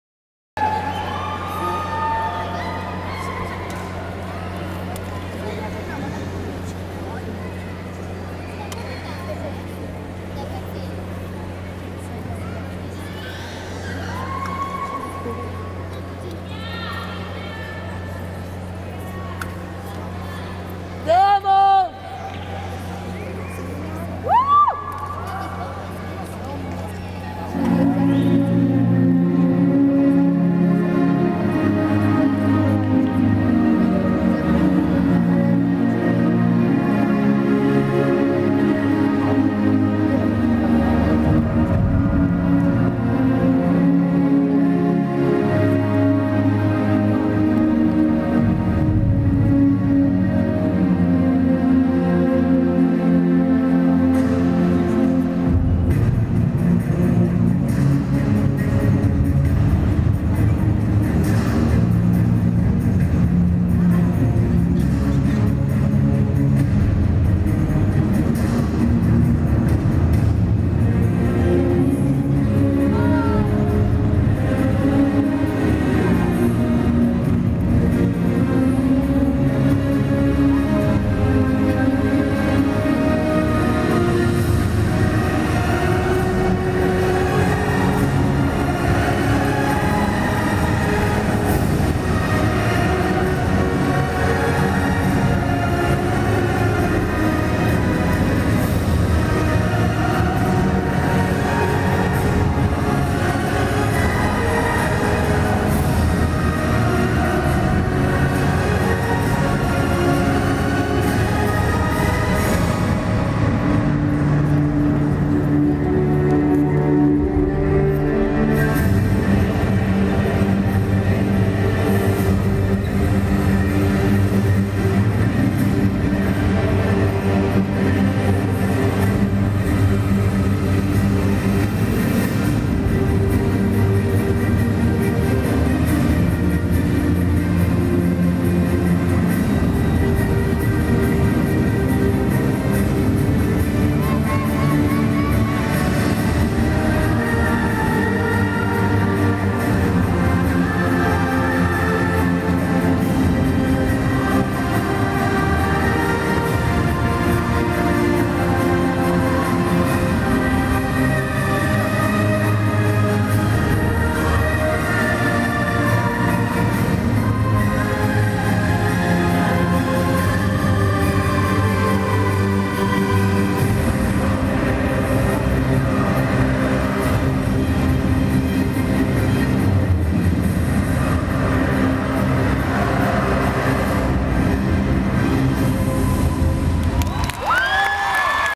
Cynthia Valdez 2 Gimnasia Rítmica
Gala Gimnástica en la Ciudad de México 2013.Gimnasia Rítmica.